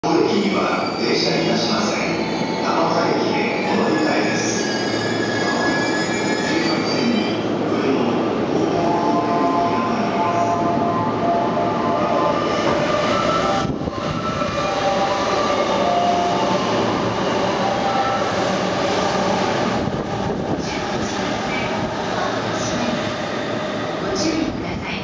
[警笛あり]JR東日本E233系1000番台150編成京浜東北線快速 日暮里駅通過
JR東日本E233系1000番台150編成京浜東北線快速の日暮里駅通過シーンです。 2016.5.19撮影